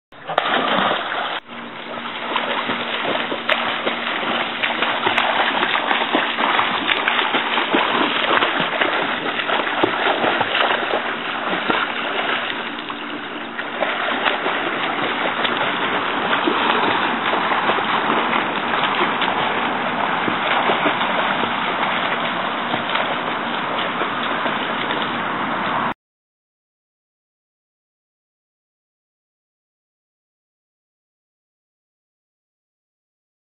My dolls swimming!
Lea and Micheala are racing eatchother! :)